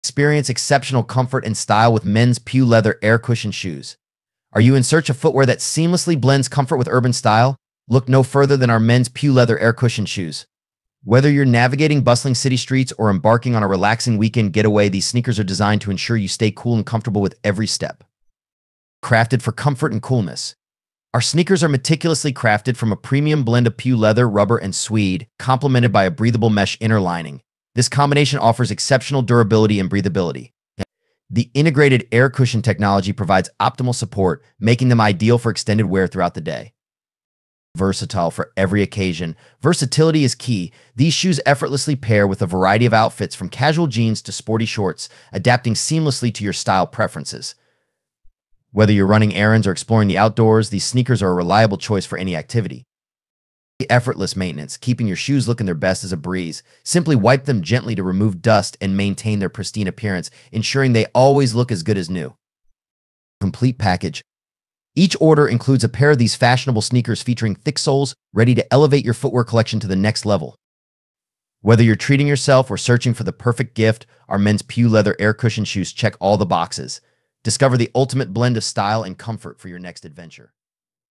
Step into Style: Men's PU Leather Air Cushion Sneakers
✔️ Similar product links. ⤸

⭐ = Step up your style game with these sleek Men's PU Leather Air Cushion Sneakers. Designed for both comfort and urban flair, these shoes are perfect for any occasion.

𝑽𝒊𝒅𝒆𝒐 𝑬𝒅𝒊𝒕𝒊𝒏𝒈: Filmora Software
𝑬𝒅𝒊𝒕𝒐𝒓: Abdullah As Sayem
𝑽𝒐𝒊𝒄𝒆: Yubair Hossain
𝑺𝒄𝒓𝒊𝒑𝒕: Atiqur Rahman

𝑪𝒐𝒎𝒑𝒖𝒕𝒆𝒓 𝒄𝒐𝒏𝒇𝒊𝒈𝒖𝒓𝒂𝒕𝒊𝒐𝒏.

𝑷𝒓𝒐𝒄𝒆𝒔𝒔𝒐𝒓: Intel(R) Core(TM) i3-6100 CPU @ 3.70GHz 3.70 GHz